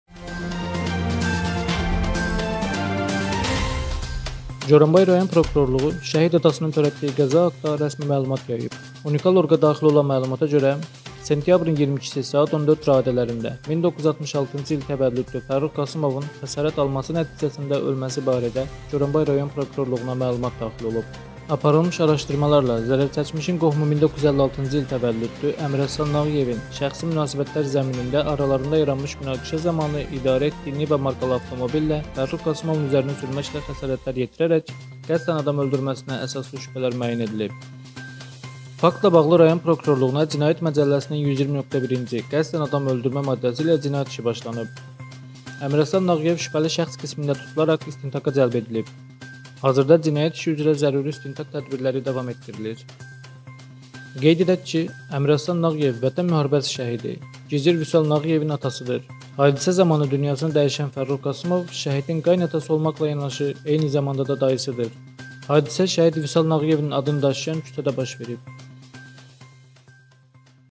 Son Dəqiqə Şəhid atası qudasına xəsarət yetirdi
Goranboy rayon prokurorluğu şəhid atasının törətdiyi qəza haqda rəsmi məlumat yayıb.

Unikal.org-a daxil olan məlumata görə, sentyabrın 22-si saat 14 radələrində 1966-cı il təvəllüdlü Fərrux Qasımovun xəsarət alması nəticəsində ölməsi barədə Goranboy rayon prokurorluğuna məlumat daxil olub.

Aparılmış araşdırmalarla zərərçəkmişin qohumu 1956-cı il təvəllüdlü Əmiraslan Nağıyevin şəxsi münasibətlər zəminində aralarında yaranmış münaqişə zamanı idarə etdiyi “Niva” markalı avtobillə Fərrux Qasımovun üzərinə sürməklə xəsarətlər yetirərək qəsdən öldürməsinə əsaslı şübhələr müəyyən edilib.

Faktla bağlı rayon prokurorluğunda Cinayət Məcəlləsinin 120.1-ci (qəsdən adam öldürmə) maddəsi ilə cinayət işi başlanıb.

Əmiraslan Nağıyev şübhəli şəxs qismində tutularaq istintaqa cəlb edilib.

Hazırda cinayət işi üzrə zəruri istintaq tədbirləri davam etdirilir.

Qeyd edək ki, Əmraslan Nağıyev Vətən müharibəsi şəhidi, gizir Vüsal Nağıyevin atasıdır. Hadisə zamanı dünyasını dəyişən Fərrux Qasımov şəhidin qayınatası olmaqla yanaşı, eyni zamanda da dayısıdır. Hadisə şəhid Vüsal Nağıyevin adını daşıyan küçədə baş verib.